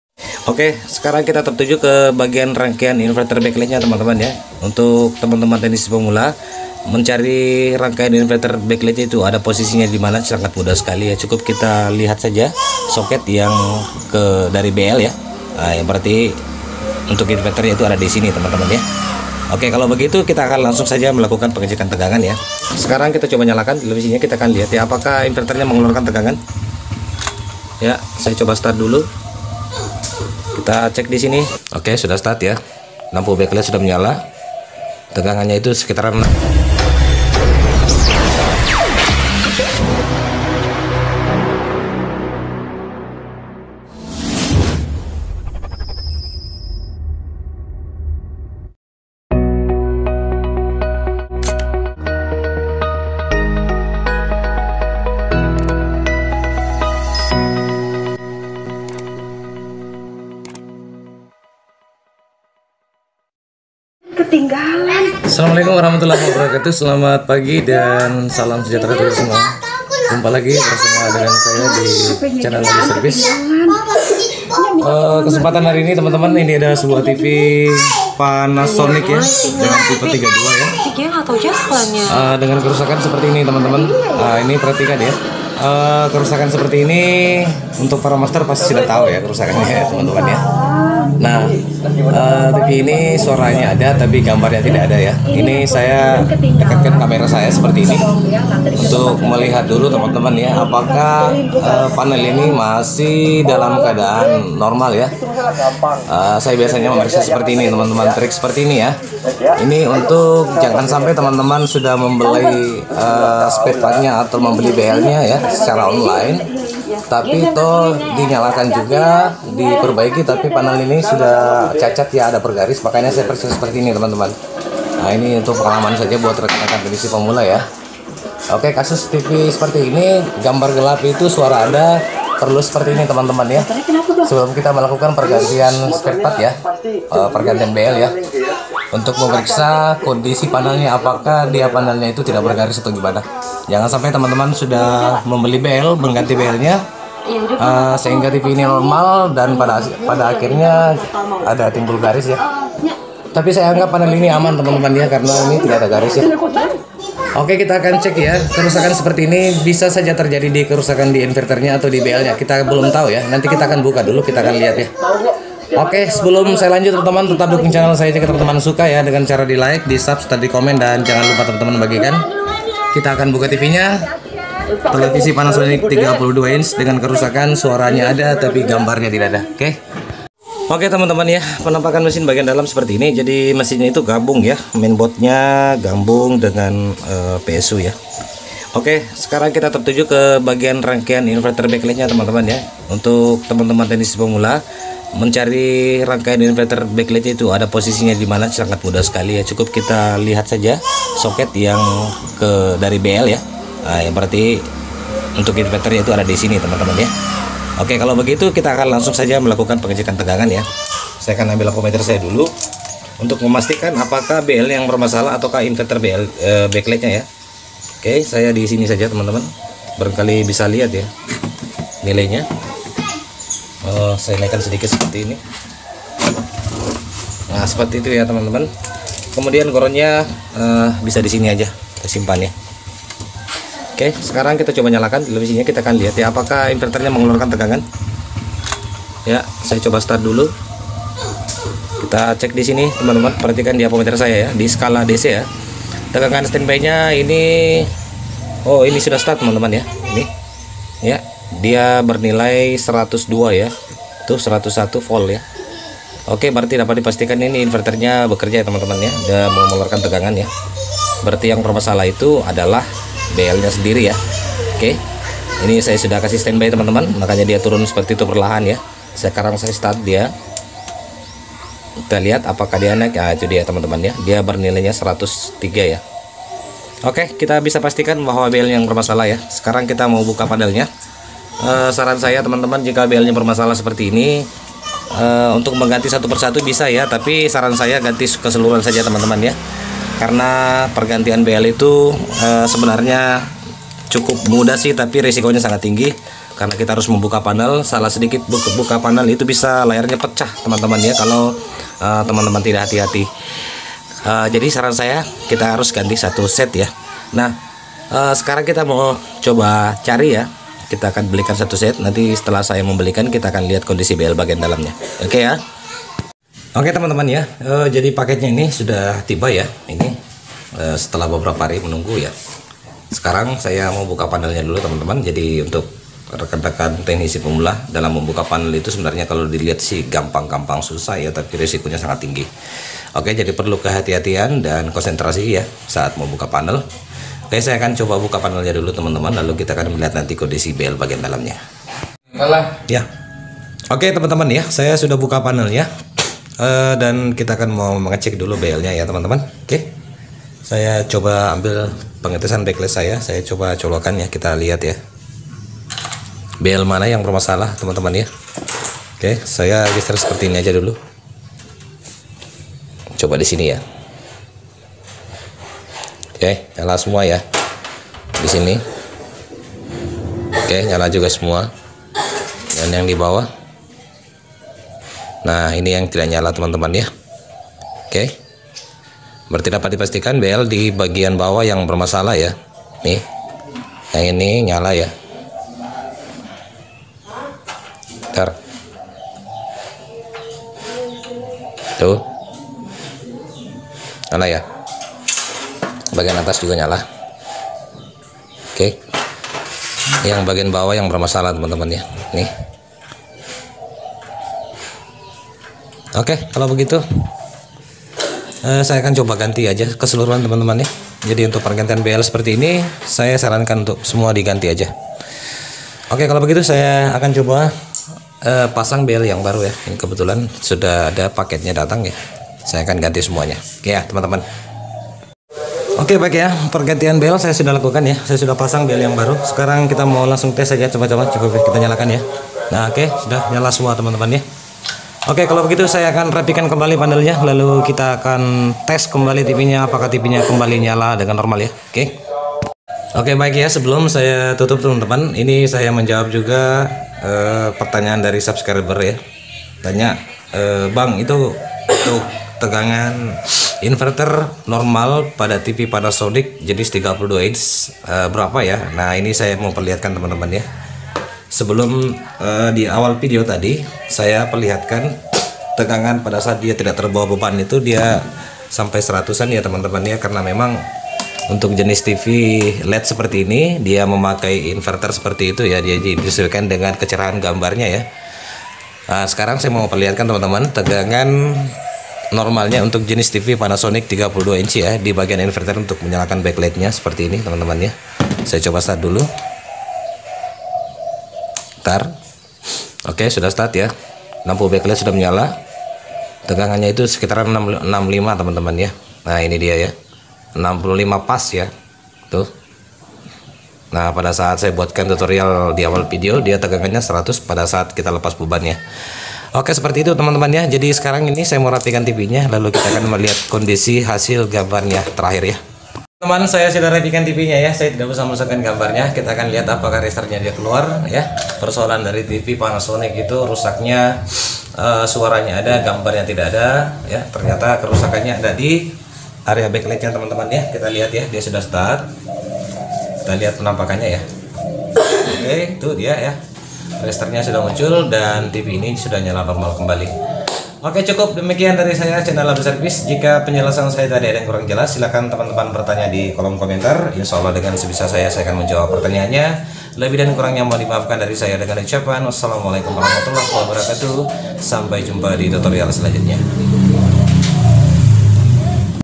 mengetahui tegangan normal inverter back laigh tv led Panasonic 32 inchi
kali ini dapat servisan sebuah tv Led Panasonic 32 inchi rusak suara ada gambar gelap kasus seperti ini biasa nya terjadi kerusakan bisa di inverter Backlaigh ataupun di Backlaigh sendiri dan di tutorial ini juga saya akan memberikan info tegangan normal inverter Backlaigh nya... ikuti tutorial nya sampai akhir video jgn di Skip Skip biar tdk gagal bagian mana kah yang rusak...? dan tetap dukung chanel ini Chanel ABI SERVIS agar saya bisa terus bersemangat dlm berbagi ilmu pengalaman dgn cara di LIKE AND SUBCRIBE